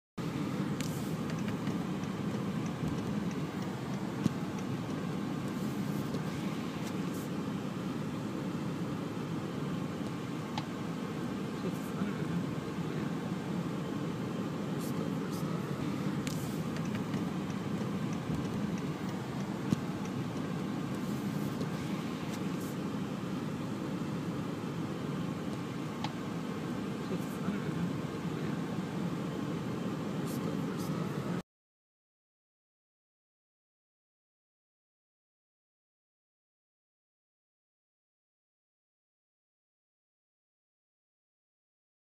Police Establish Random Checkpoint On Road Outside Austin Airport
We see the police along with the TSA are encroaching further on our Constitutional right to travel. Outside Austin-Bergstrom International Airport local Police have established a random checkpoint. Our reporter shot this footage as he was leaving the Austin airport on July 17th. Now we see the TSA all over America, at bus stations, train stations, sporting events, concerts, and even shopping malls. This will not stop unless we stand and speak out. Papers please.